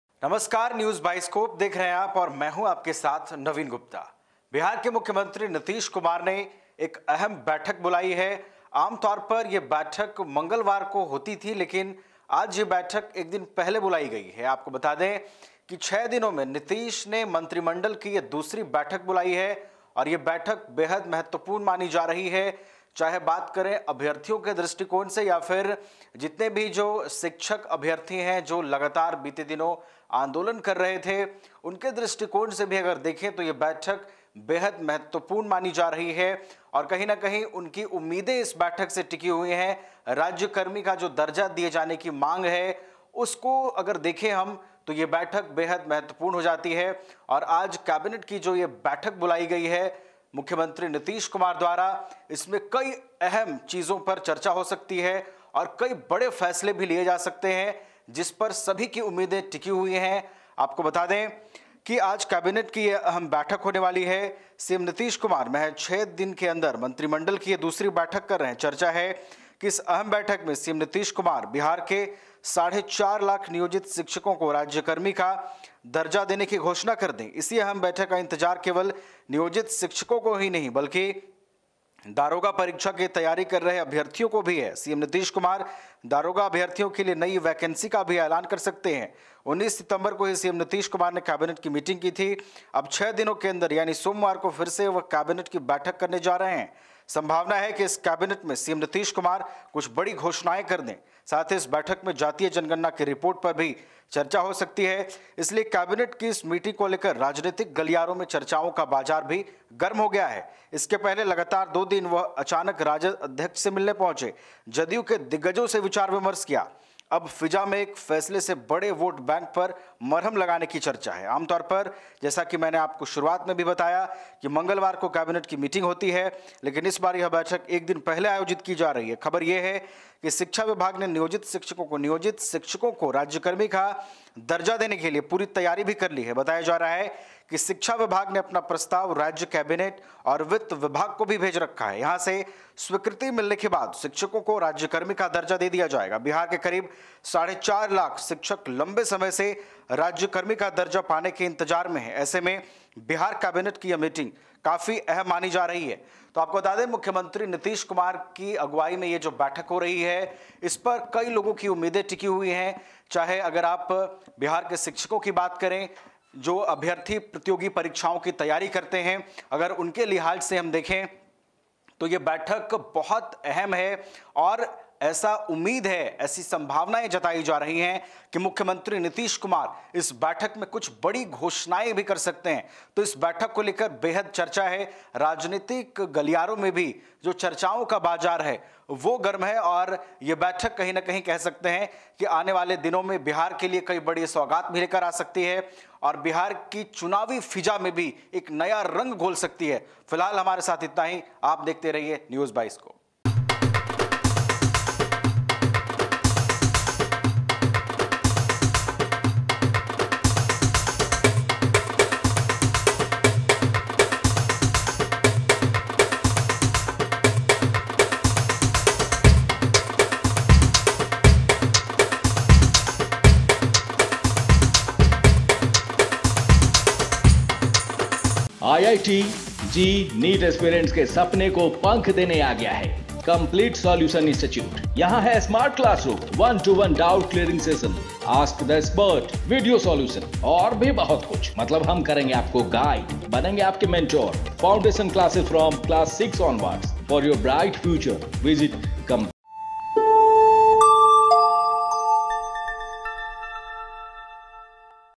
नियोजित शिक्षकों को बिहार कैबिनेट से मिलेगी सौगात, सालों की मांग पर सीएम नीतीश लगायेंगे मुहर!
Employed teachers will get a gift from Bihar Cabinet, CM Nitish will seal their demand for years!

About this Video: -नियोजित शिक्षकों को बिहार कैबिनेट से मिलेगी सौगात, सालों की मांग पर सीएम नीतीश लगायेंगे मुहर!

22Scope Live TV | 22 Scope | Hindi News | 22 Scope लाइव

CHANNEL AVAILABLE
JIO TV
VI
ROCK TV